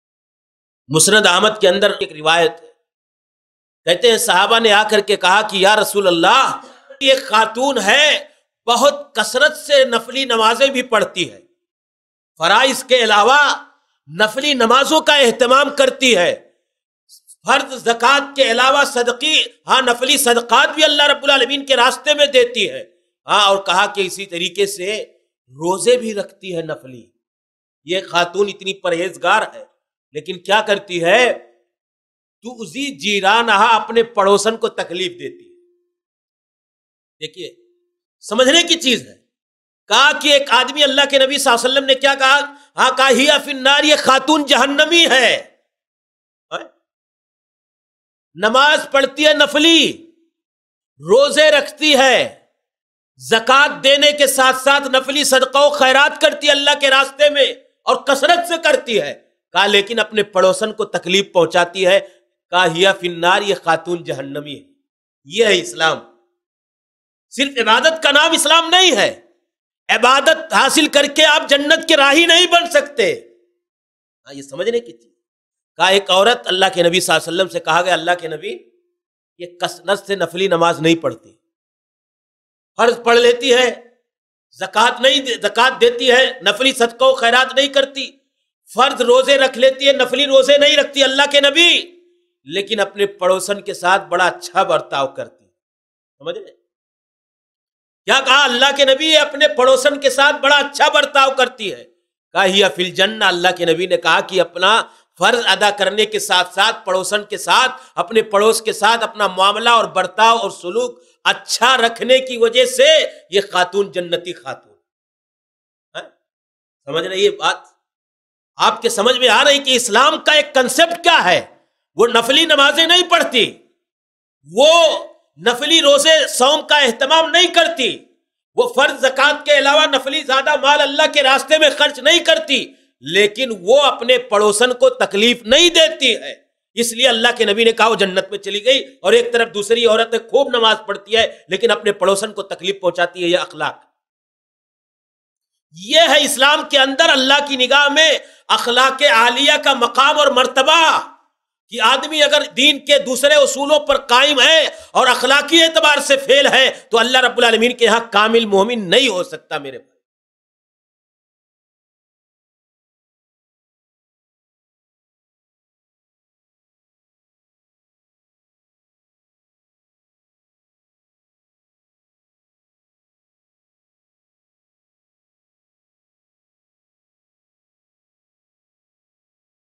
Ek Ibaadat Guzaar Aurat Ka Jahannami Hone Ka Waqeya By Shaikh Aatif Sanabili
Ek Bahot Ibaadat guzaar Aurat Akhlaq Kharab Hone Ki Wajah Se Jahannami Huyi By Shaikh Aatif Sanabili